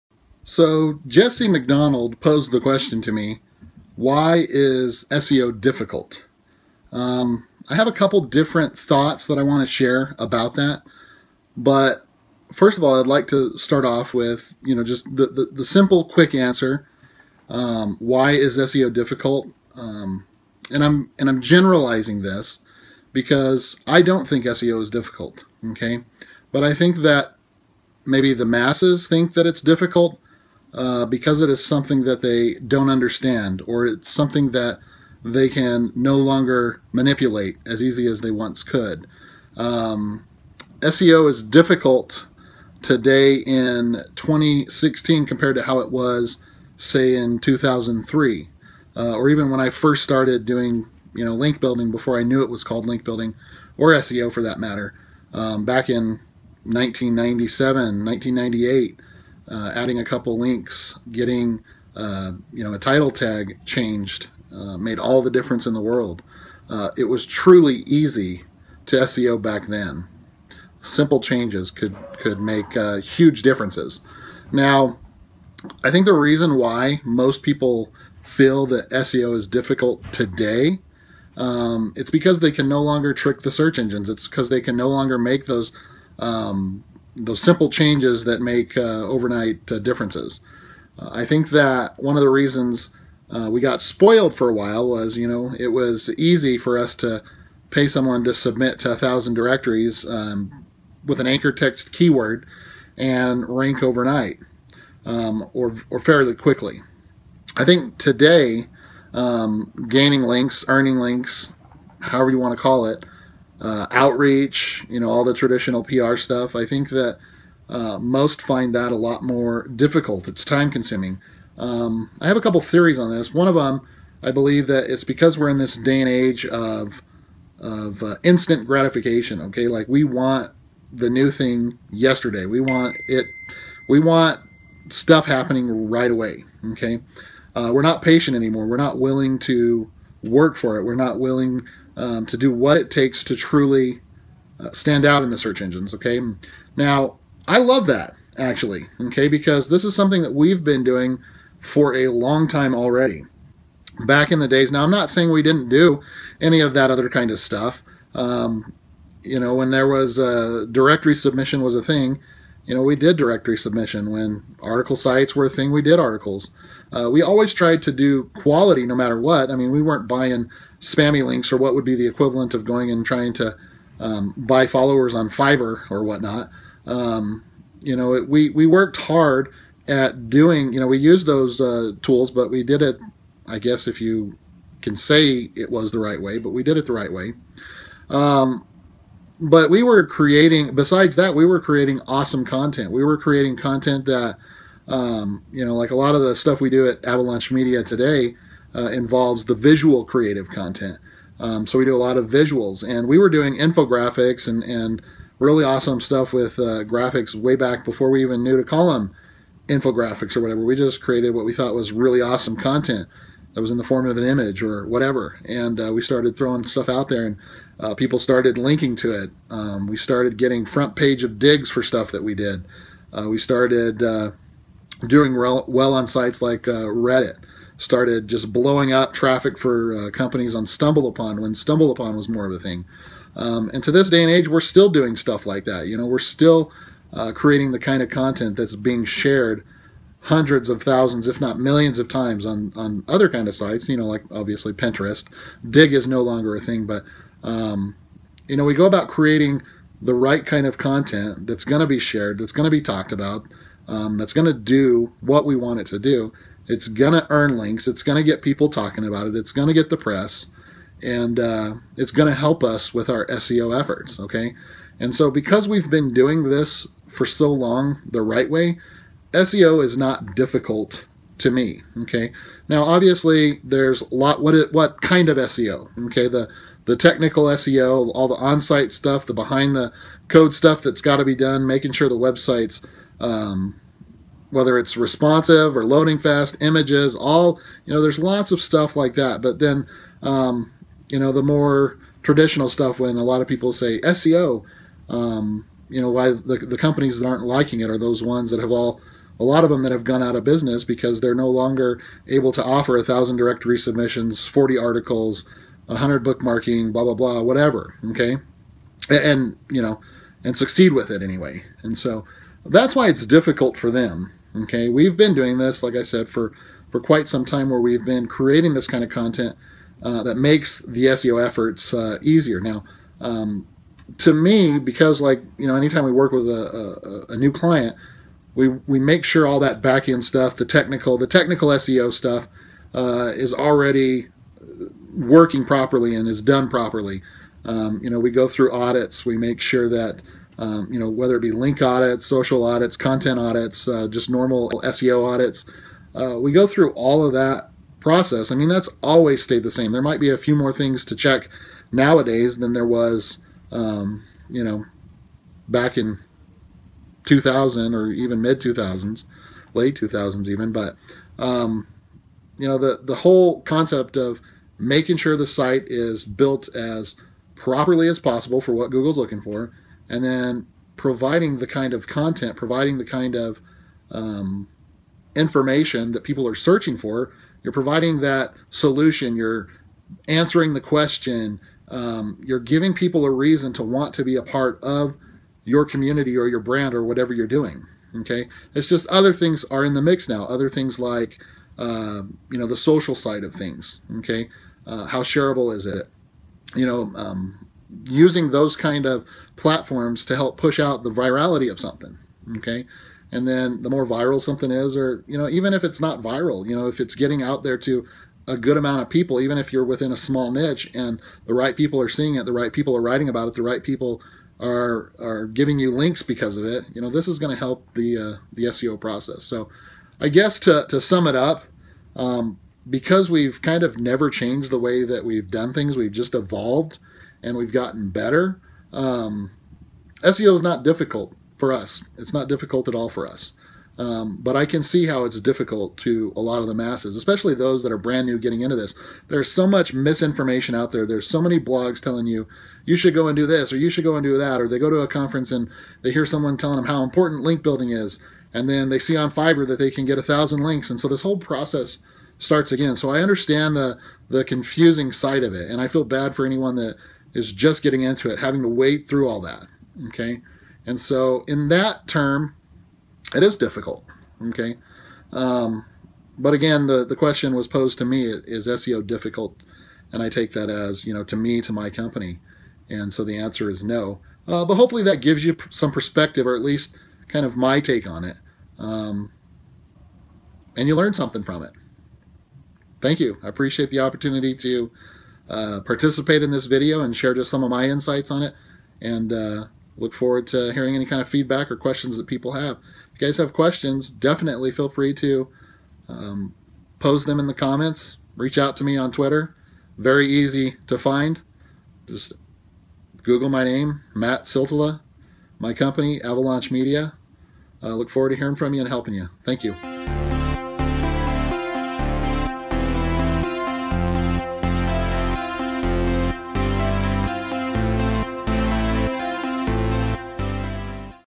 Matt Siltala - Why is SEO Difficult? - Geek Powered Studios
Matt Siltala, President and Founder of Avalaunch Media, gives his views on why he thinks SEO is difficult.